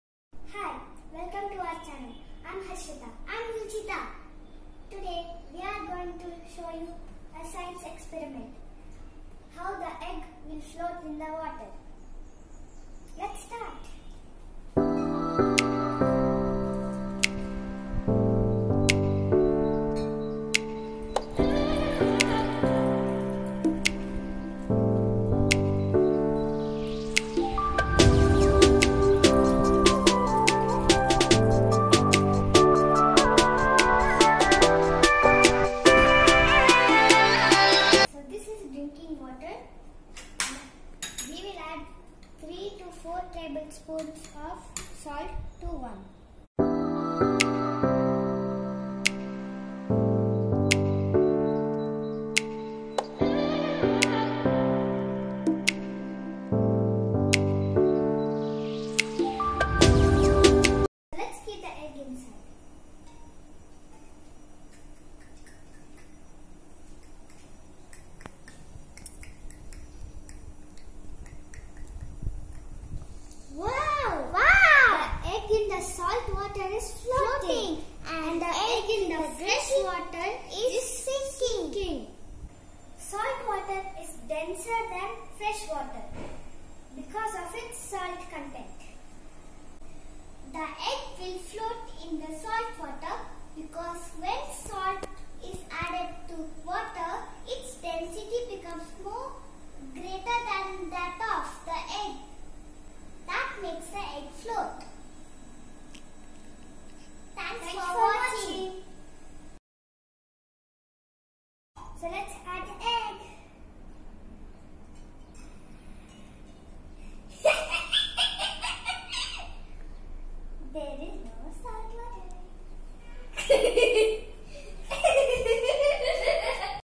Science experiment: Why the Egg floats in Salt Water!
Fun with Science facts